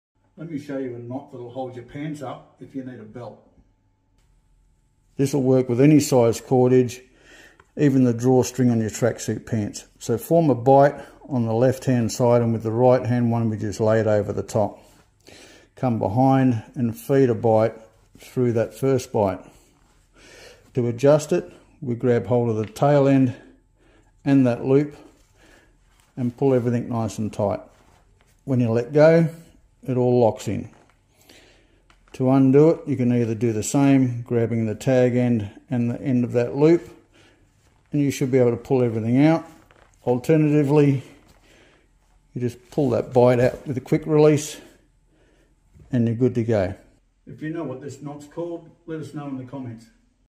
How to tie an adjustable knot that will hold your pants up. Works on gym shorts, track pants or belt
How to tie a rope to hold your pants up. Will work on gym shorts, track pants and an improvised rope belt.